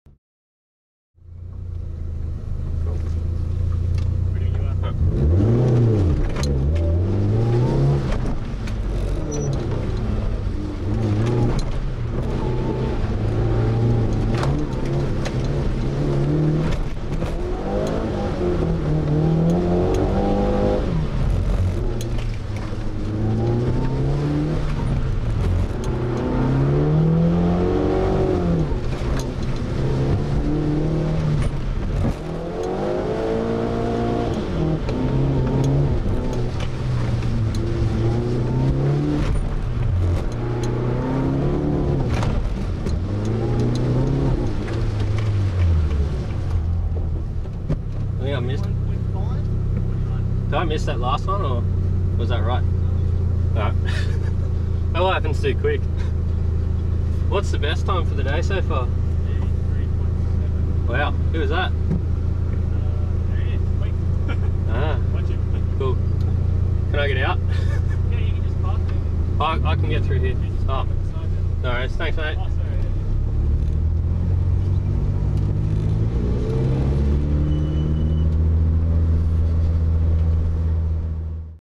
Eastern Creek Skid Pan - Mitsubishi Evo 7
Eastern Creek Skid Pan - Mitsubishi Evo 7. Footage from the 2011 EvolutionOz EvoNats